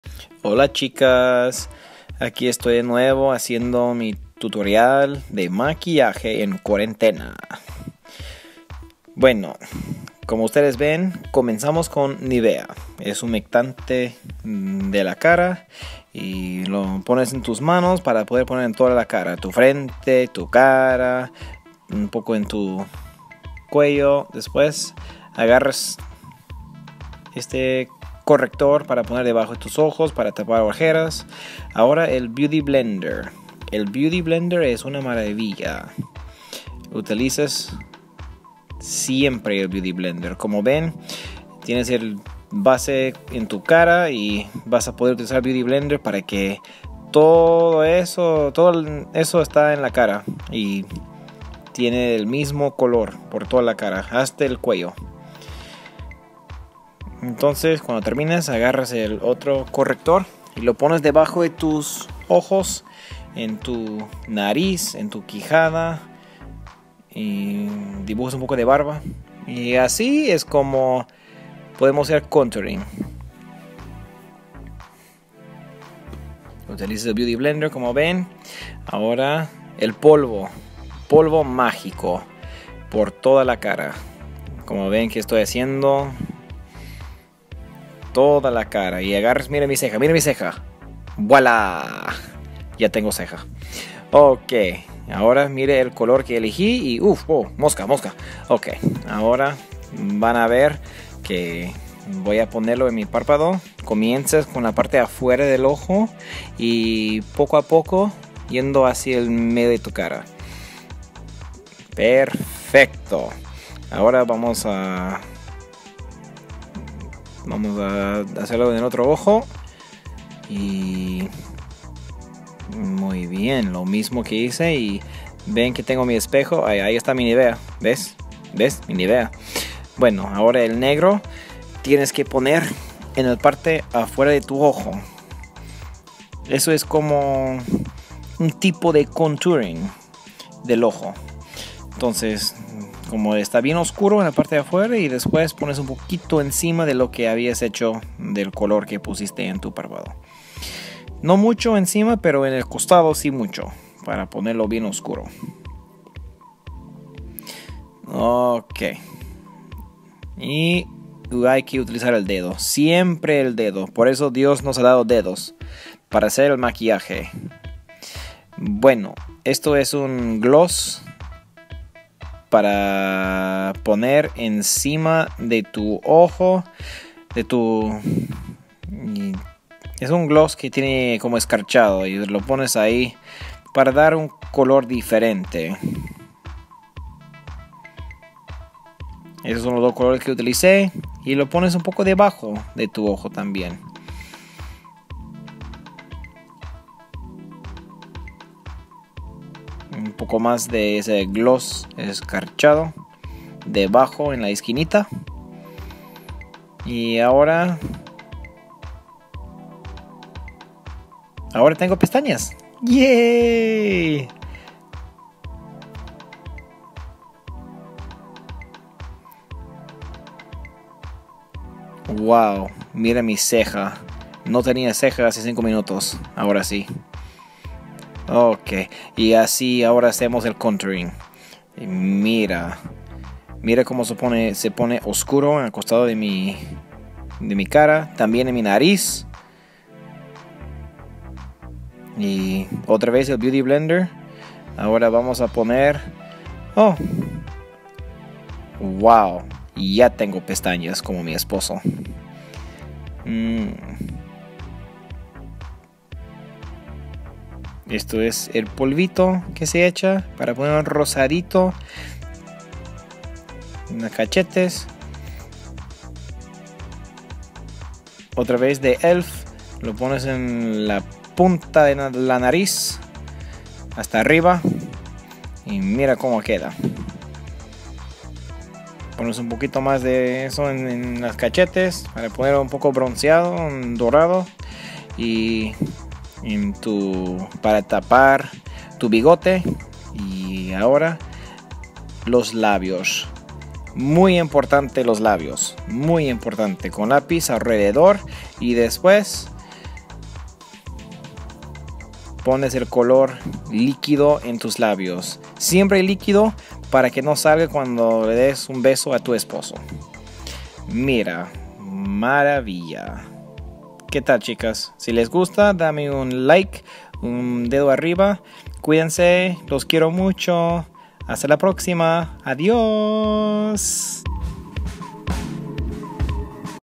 MI HERMOSO ESPOSO NARRA MI VIDEO | slowlybutshirley
Maquillaje de cuarentena para ir a la sala. My husband does my voiceover. Video super gracioso,
Tutorial de Maquillaje fácil para fiesta, cumpleanos y/o para ocasión especial. Maquillaje escarchado.

Espero que lo hayas disfrutado!
Dale "ME GUSTA" y COMPARTELO!

Abrazos de oso para todos!
SlowlyButShirley (LentoPeroSeguro) xoxo